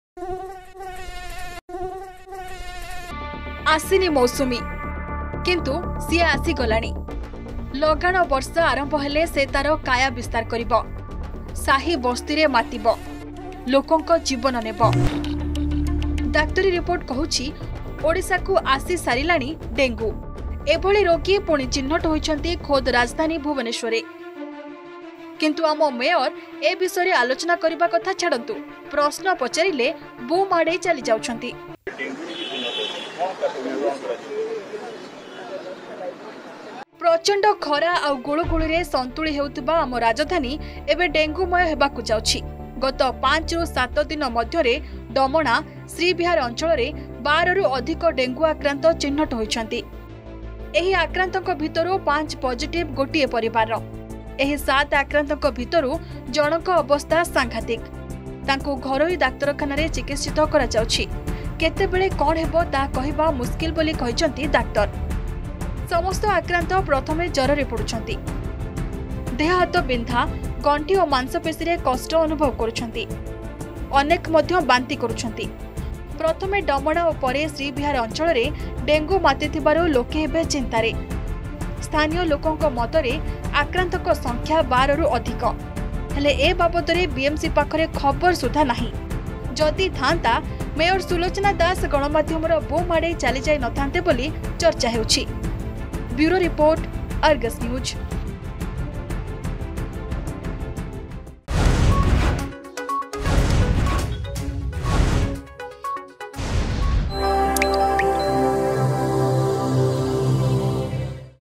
ସ୍ମାର୍ଟସିଟିରେ କାୟା ମେଲାଉଛି ଡେଙ୍ଗୁ
ସ୍ମାର୍ଟସିଟିରେ କାୟା ମେଲାଇଲାଣି ଡେଙ୍ଗୁ । ଲଗାଣ ବର୍ଷା ଆରମ୍ଭ ହେଲେ ସେ ତାର କାୟା ବିସ୍ତାର କରିବ । ସାହି ବସ୍ତିରେ ପସିବ ଓ ଲୋକଙ୍କ ଜୀବନ ନେବ । ଆଉ ଡାକ୍ତରୀ ରିପୋର୍ଟ କୋହୁଛି ଓଡ଼ିଶାରେ ଆସି ସାରିଲାଣି ଡେଙ୍ଗୁ ।